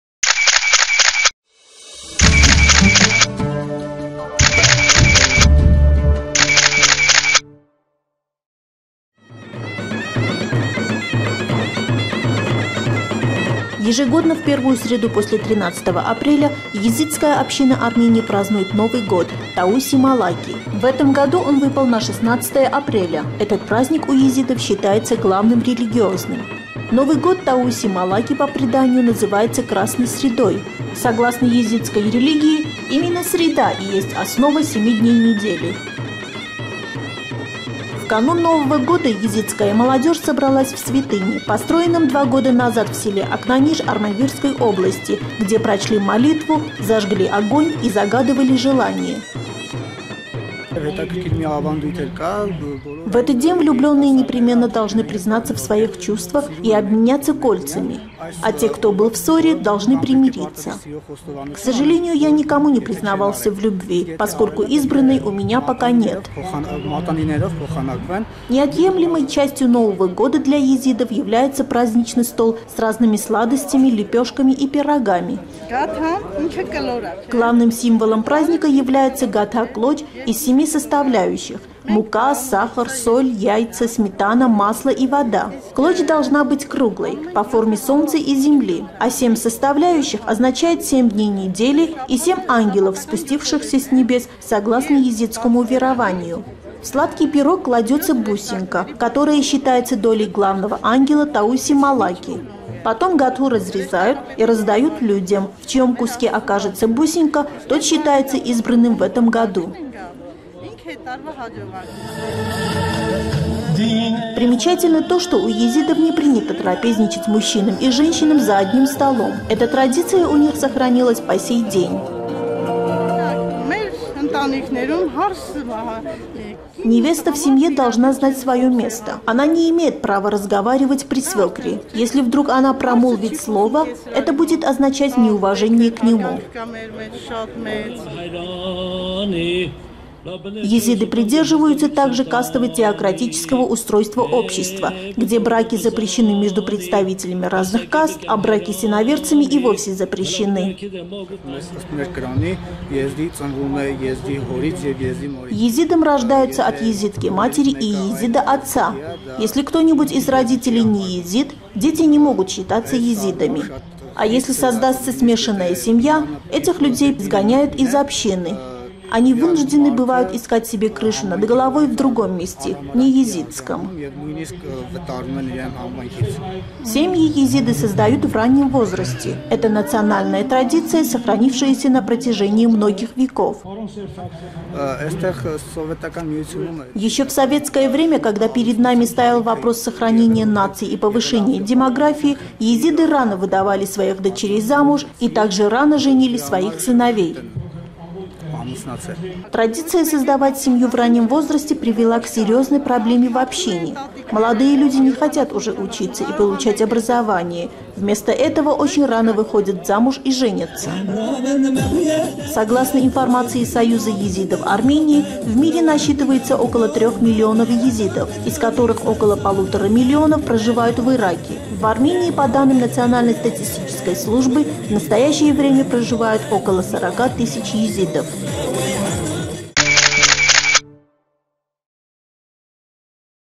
Yazidis celebrate New Year (Armenia)
Every year, on the first Wednesday after April 13, the Yazidi community of Armenia celebrates the New Year -- Tawusi Melek. This year it fell on April 16. This holiday is the Yazidi's major religious celebration. It is also called "Red Wednesday", since the Yazidi believe Wednesday to be the main day of the week.